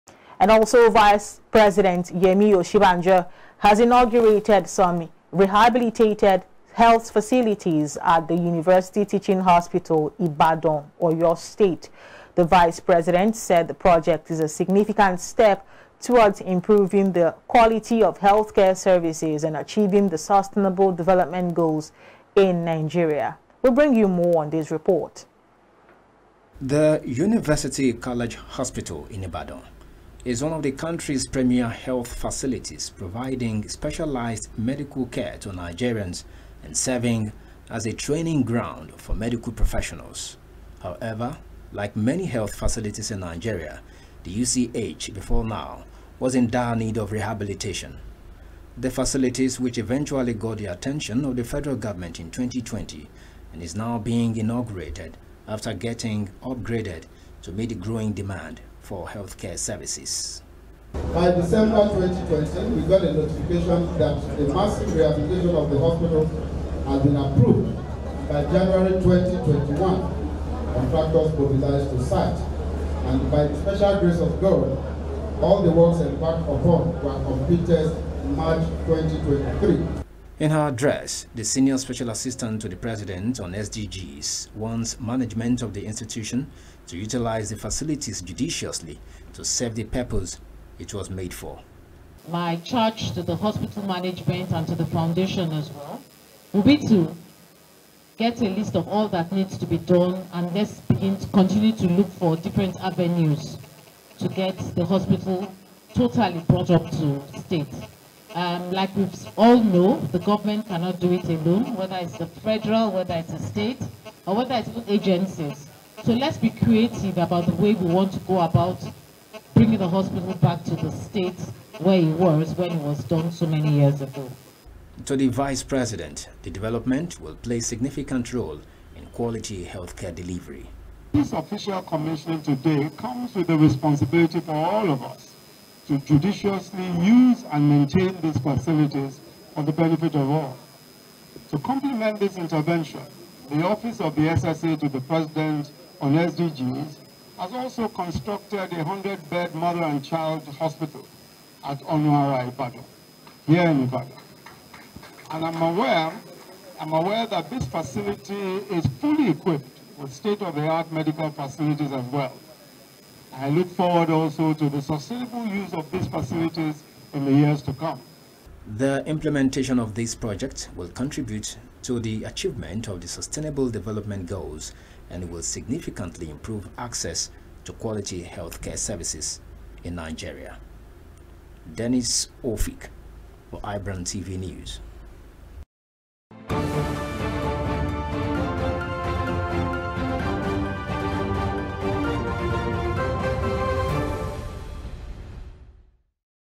VICE-PRESIDENT OSINBAJO INAUGURATES REHABILITATED HEALTH FACILITY IN IBADAN
Nigeria’s Vice President, Prof, Yemi Osinbajo on Tuesday inaugurated the rehabilitated sections of the University College Hospital, Ibadan, Oyo State, Southwest Nigeria.

The project was midwife and driven by the Office of the Senior Special Assistant to the President on the Sustainable Development Goals, SDGs.